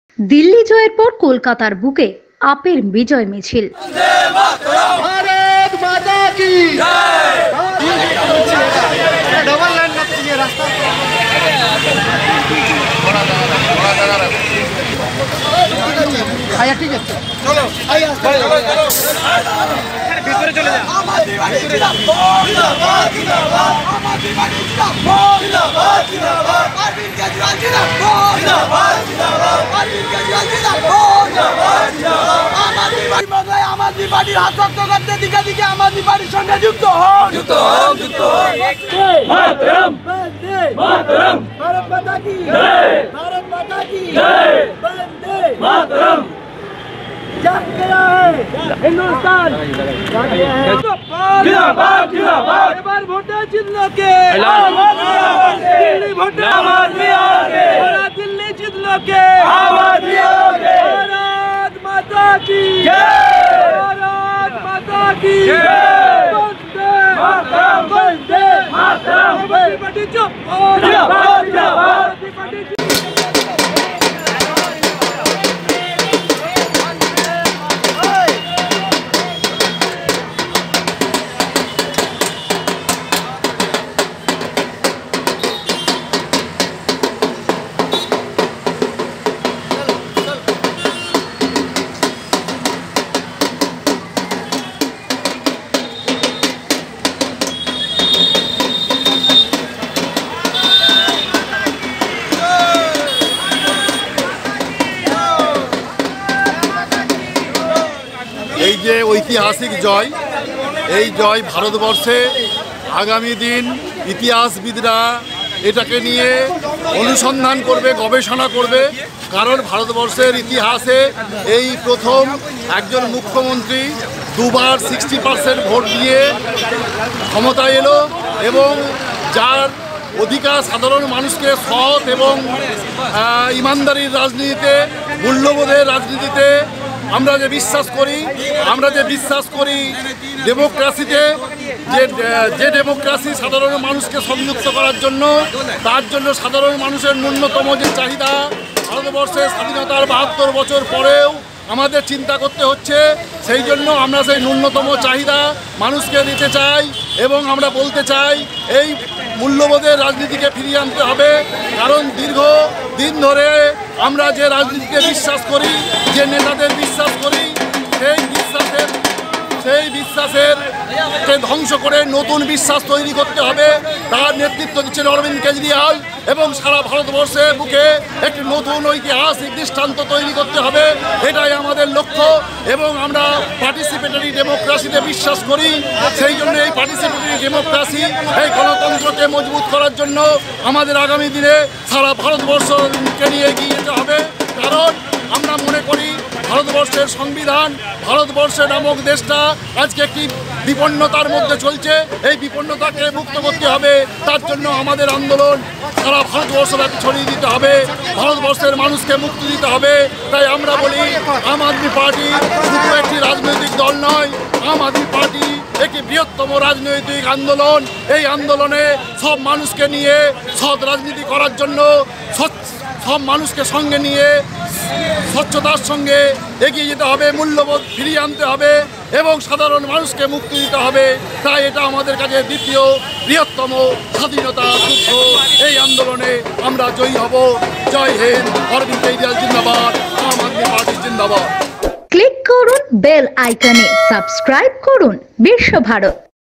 দিল্লি জয়ের পর কলকাতার বুকে আপের বিজয় মিছিল।biswa bharat
দিল্লি জয়ের পর কলকাতার বুকে আপের বিজয় মিছিল।